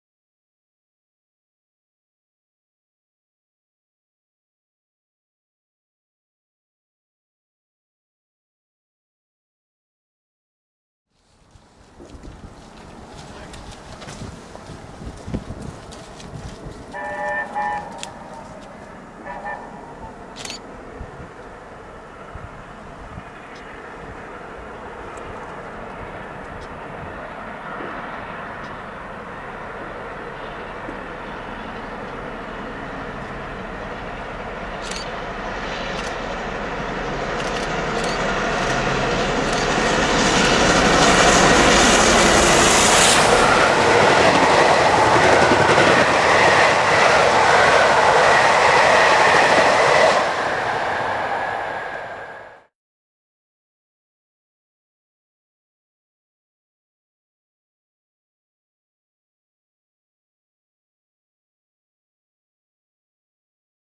60019 'Bittern' Hauls The Cathedrals Express Through Cholsey 05/12/2009
The 5th day of the Christmas month brings 60019 'Bittern' to haul The Cathedrals Express Charter past Cholsey. As she was booked to go straight through Didcot Parkway, there certainly didn't seem any messing about from her on the straight at Manor Farm, Cholsey. Quite a nice powerful performance in all fairness from the LNER A4 Class steam locomotive.
Locomotive - LNER A4 Class 4-6-2 No.60019 'Bittern'
Route - London Paddington - Cardiff - London Paddington
Steam Hauled Throughout
Location - Manor Farm, Cholsey
Date - 05/12/2009